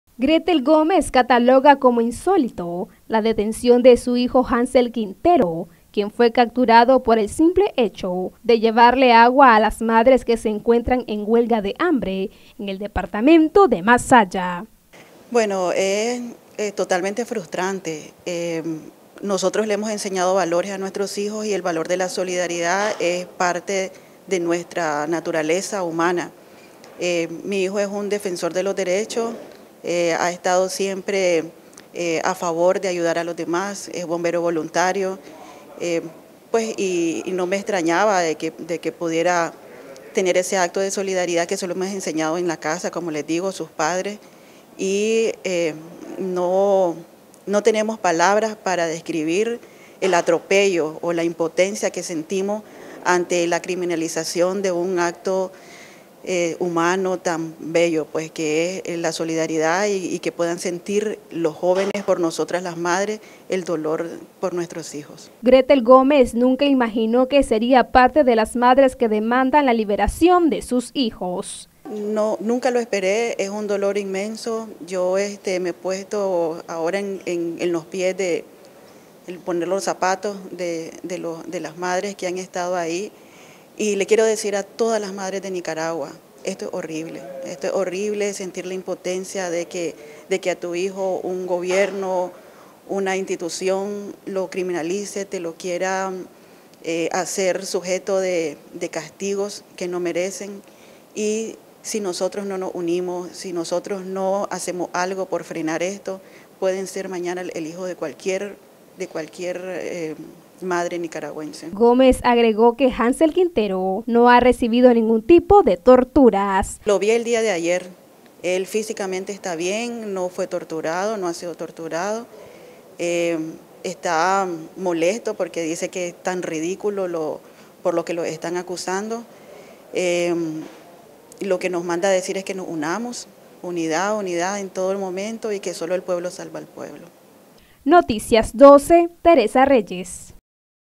GÓMEZ: ES INSÓLITO SER ENCARCELADO POR SER SOLIDARIO
Grethel  Gómez Madre del preso político Hansell Quintero demando la pronta liberación de su hijo quien fue detenido por el simple hecho de llevarles agua a las madres que se encuentran en huelga de hambre en Masaya.